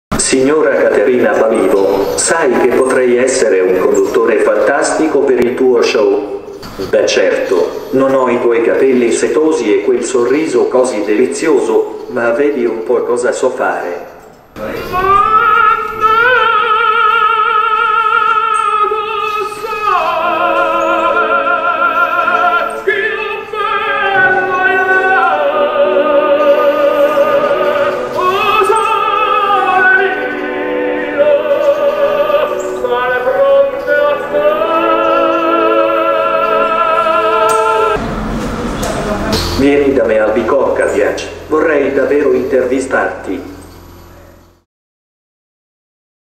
Robot invita Caterina Balivo alla “Città dei Robot”
Thespian invita Caterina Balivo alla “Città dei Robot”, a Milano dal 12 dicembre 2019 al 9 febbraio 2020.